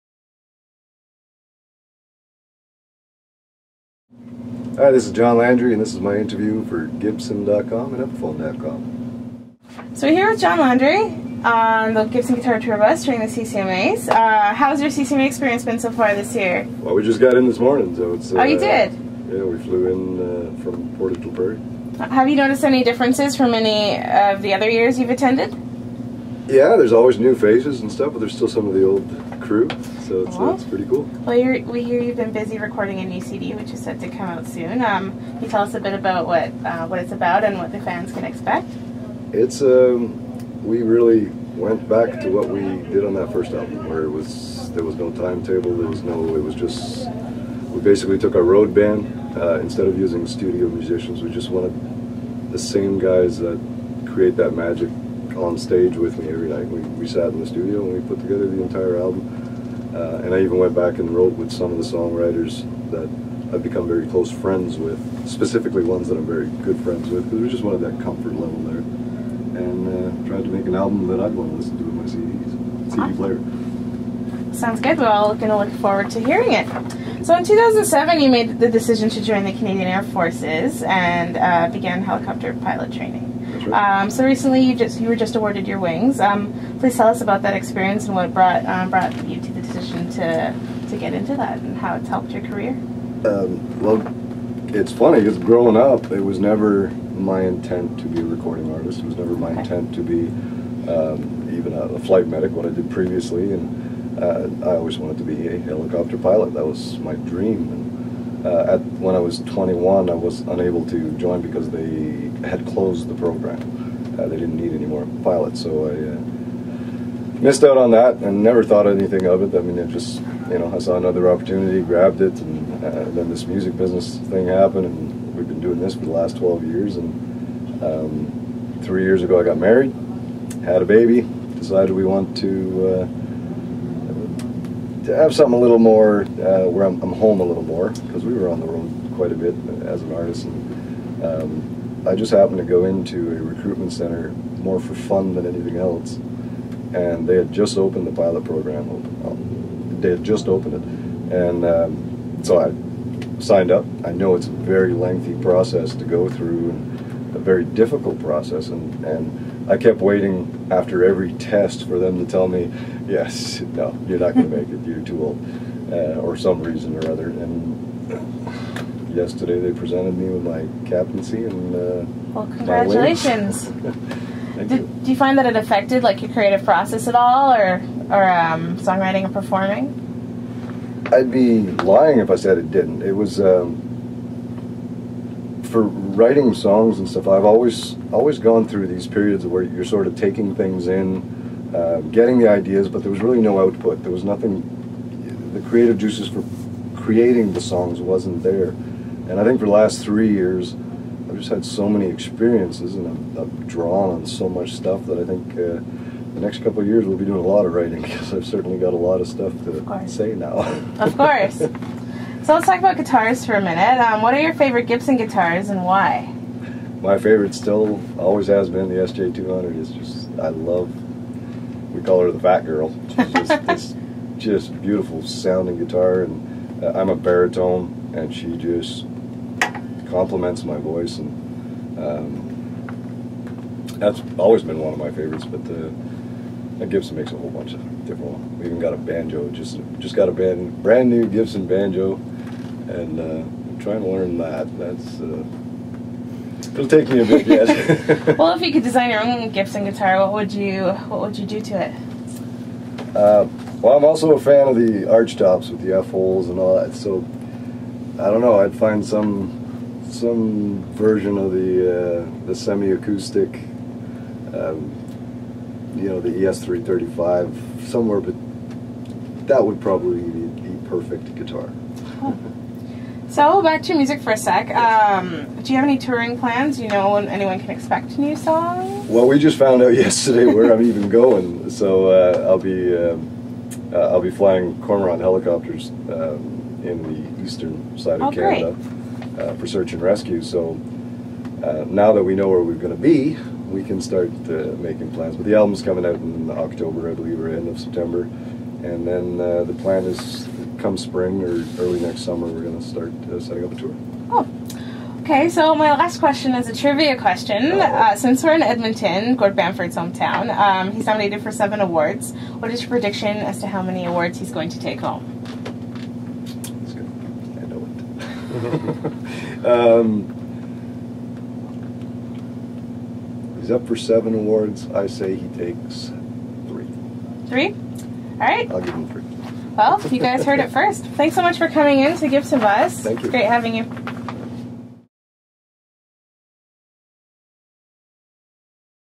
Gibson - John Landry Interview @ The 2010 CCMA's
interview with John Landry on the Gibson Guitar tour bus at the 2010 CCMA's in Edmonton.